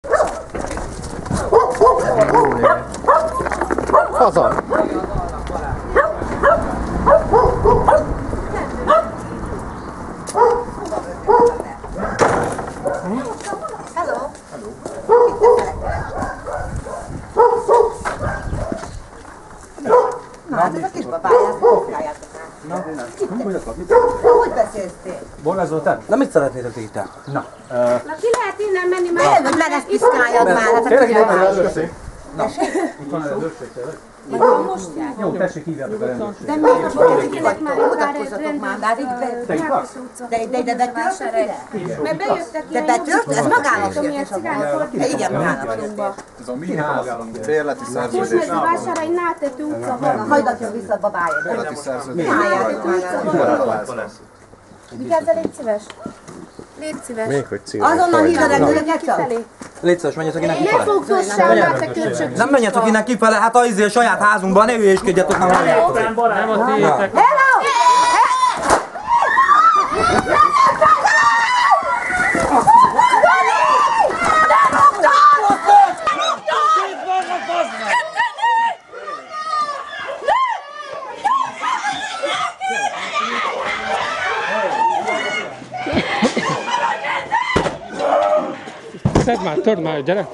A padlóla, de dulván!
A Tolvajkergetők Hódmezővásárhelyen próbáltak birtokba venni egy ingatlant, amelyből Borbás Zoltán bűnöző kikergette jogos tulajdonosát, az idős Benyhe Lászlónét. A városszerte rettegett, agresszív bidámbidám azt hitte, viccelünk. Így futnak a bűnözők a tisztességes emberek elől, ha összefogunk!

Javított változat, ezen már látszik, hogyan menekül be a lakásba.

Több újság is torzan és a bűnöző védelmében fellépve közölte a történteket, azt sugallva, hogy egy békés családra tört rá "önbíráskodó" társaságunk. Ez hazugság, és az ezt állító lapok ellen pert indítunk. A történet teljes háttere, a valóság itt elolvasható: